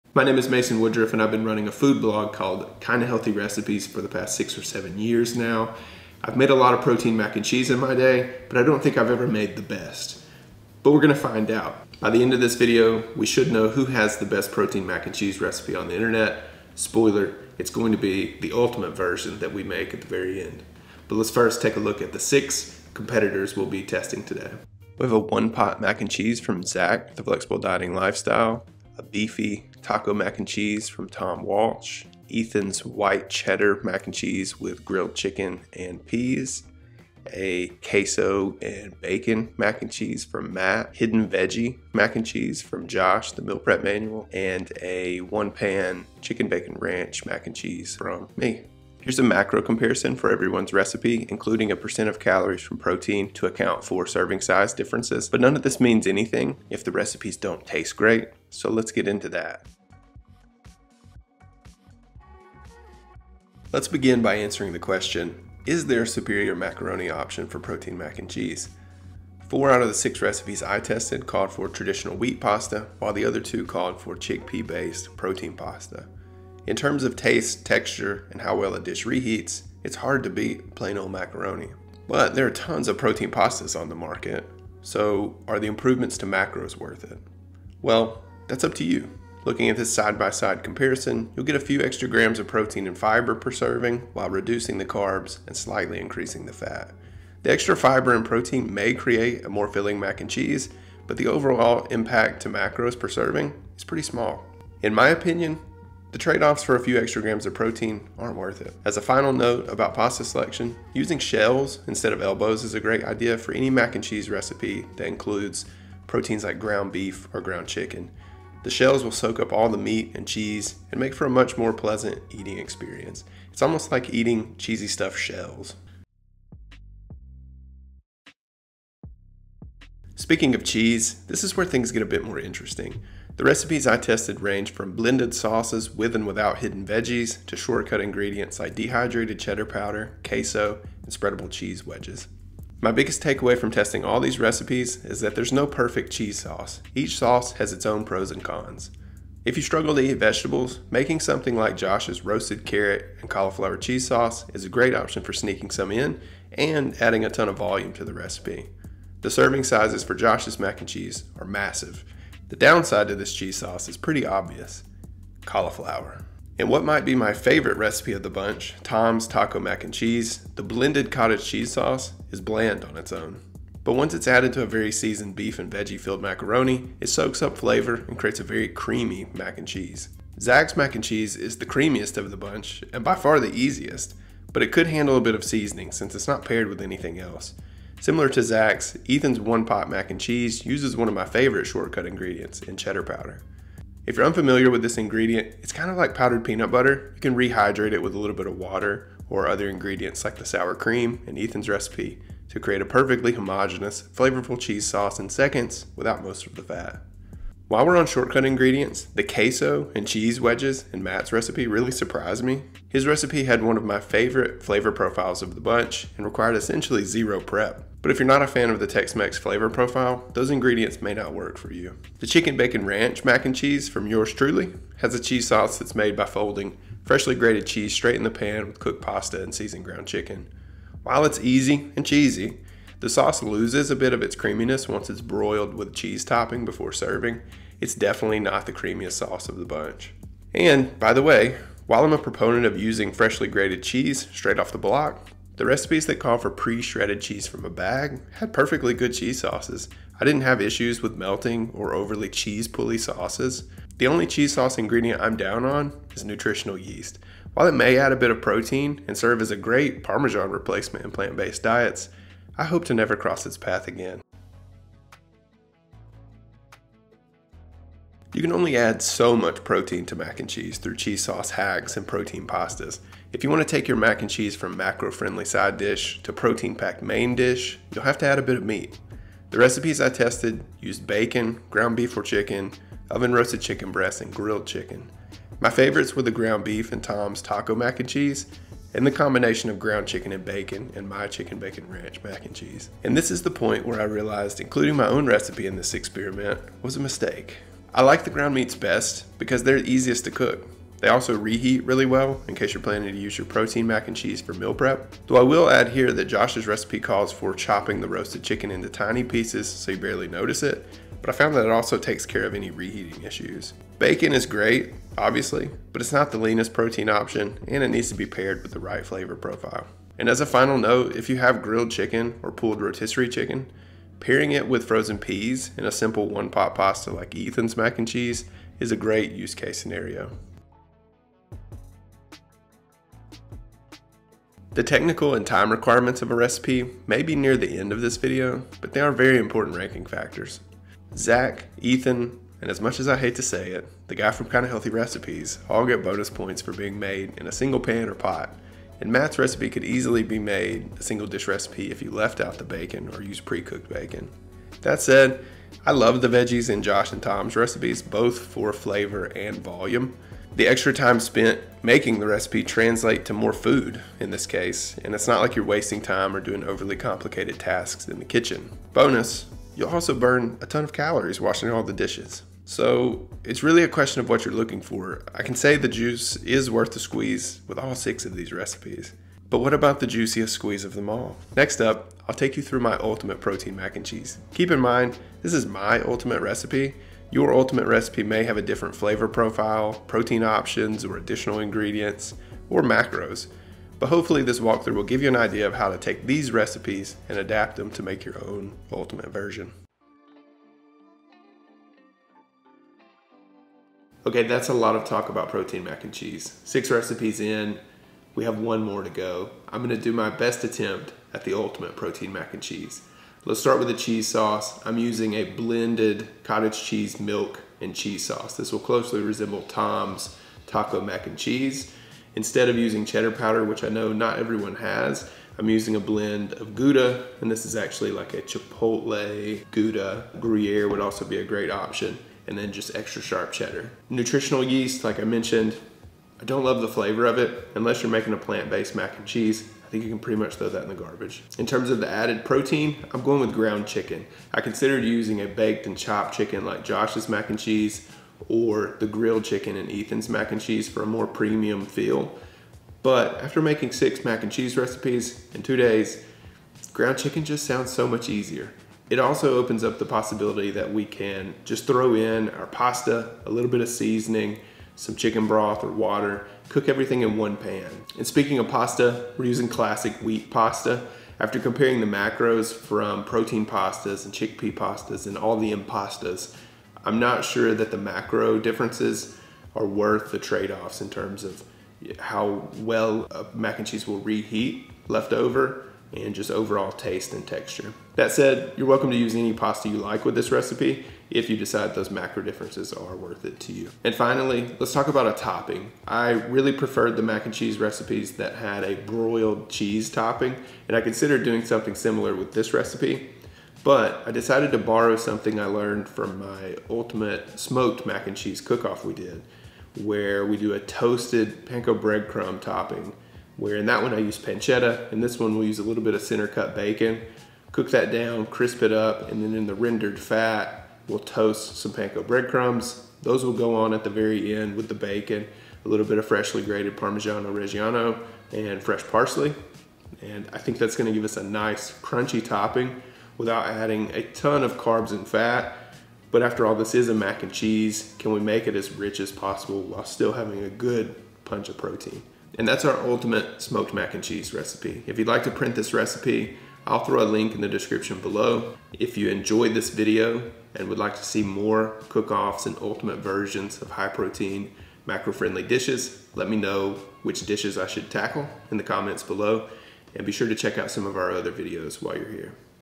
I Found the Best Protein Mac and Cheese Recipe
I've made tons of protein mac and cheese in my seven years of food blogging, but I wanted to see how my recipes stacked up to other heavy hitters. So I tested some of the best high protein mac n cheese recipes you can find online to find out.

I also used my findings from this experiment to create MY ultimate version at the end of the video.

Timecodes:
0:00 Intro
0:24 The Six Recipes
0:48 Comparing Macros for Each Recipe
1:03 What's the best pasta for protein mac and cheese?
2:09 High Protein Cheese Sauce
4:27 Added Proteins
5:34 Comparing the Difficulty of Each Recipe
6:43 Making My Best Protein Mac and Cheese